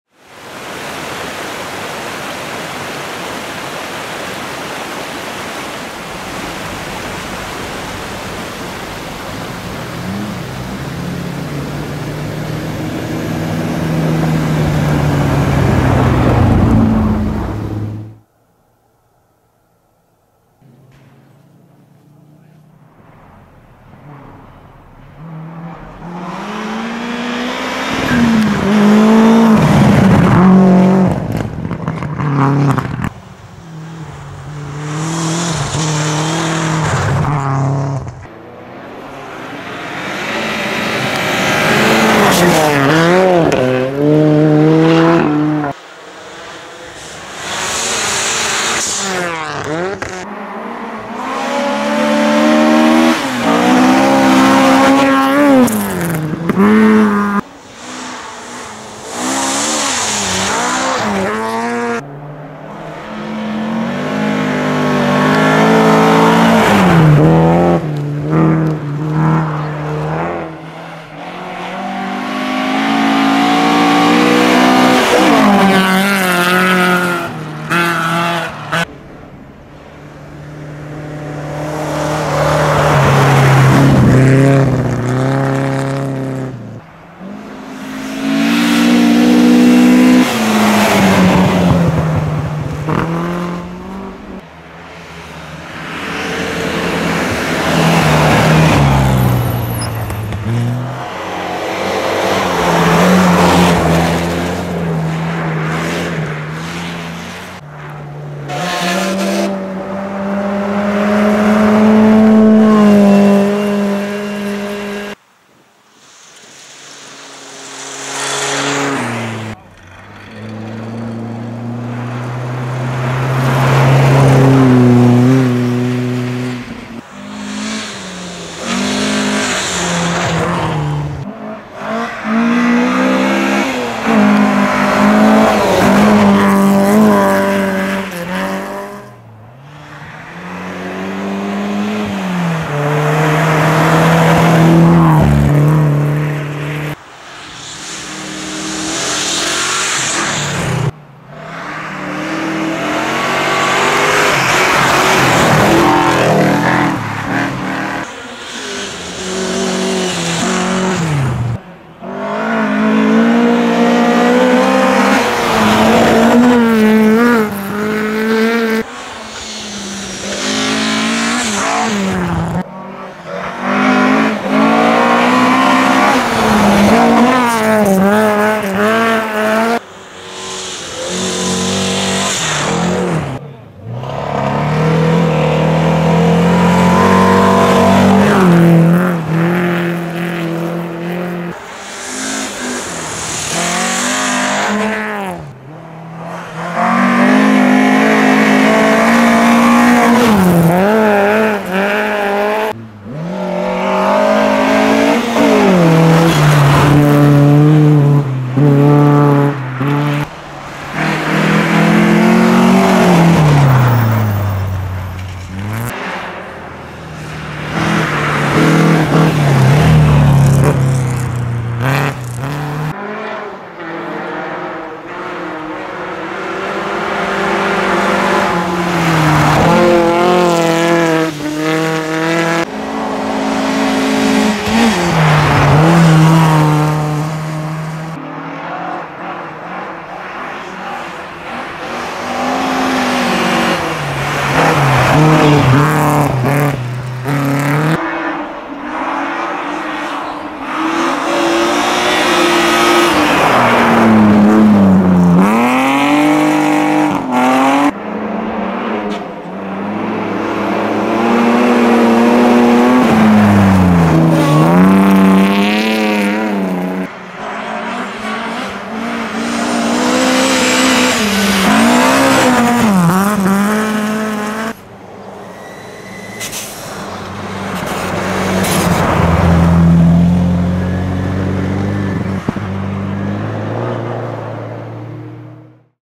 HRT Kuhmoisten Sora-Ralli 2022
Historic Rally Trophy Kuhmoinen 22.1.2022 Round 1
Kuhmoisten Sora-Ralli 2022
Kuvauksia Päijälästä EK1